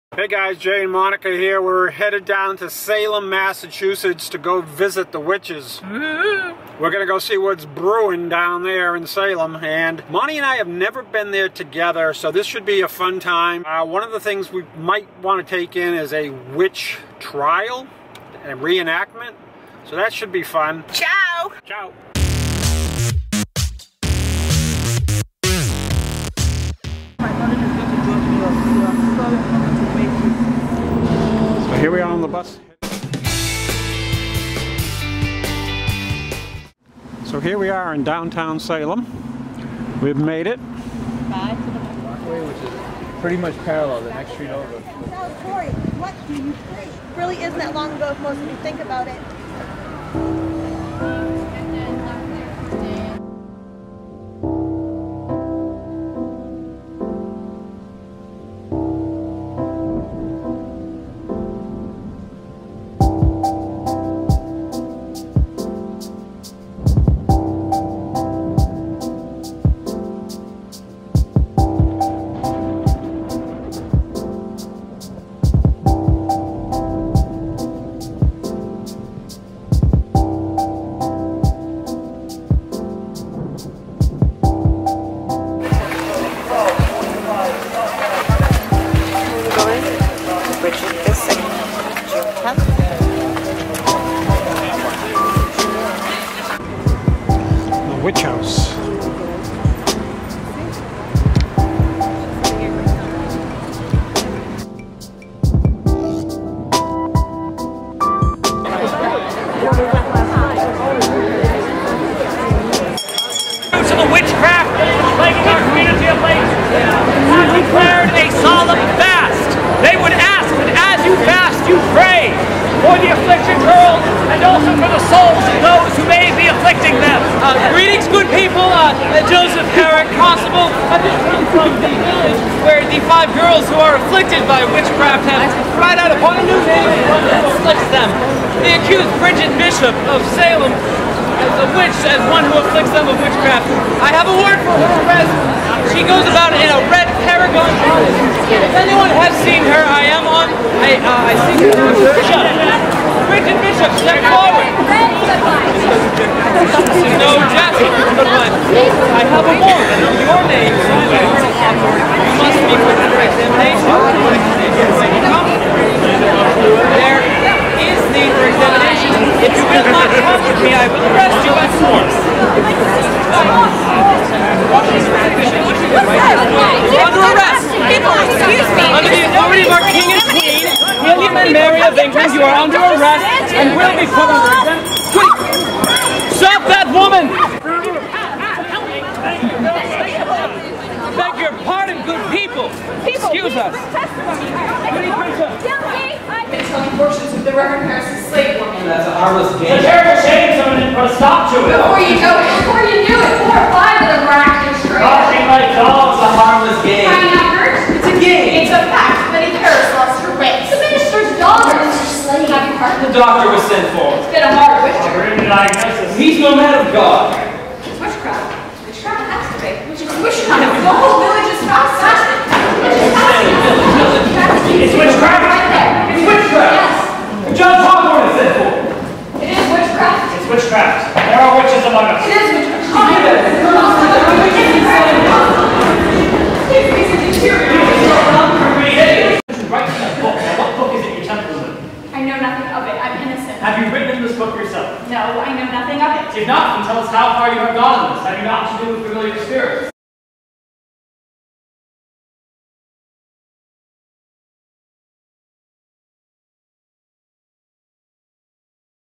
Witch Trial Bridget Bishop Salem, MA
A quick video of our trip to Salem, MA on Halloween weekend.
Jay McGillicuddy
Berkshire Hathaway Verani Realty